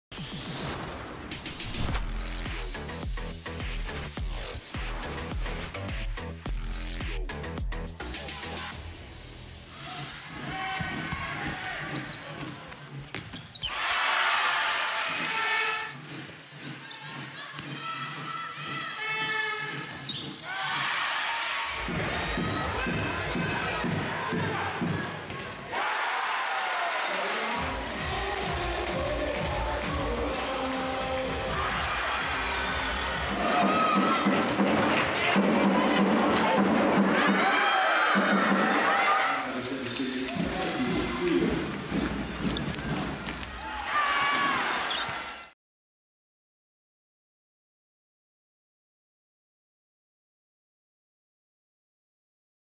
Bambusa Ugens redninger (10)
Ingen uge i Bambusa Kvindeligaen uden en perlerække af redninger fra målvogterne – nyd de allerbedste her.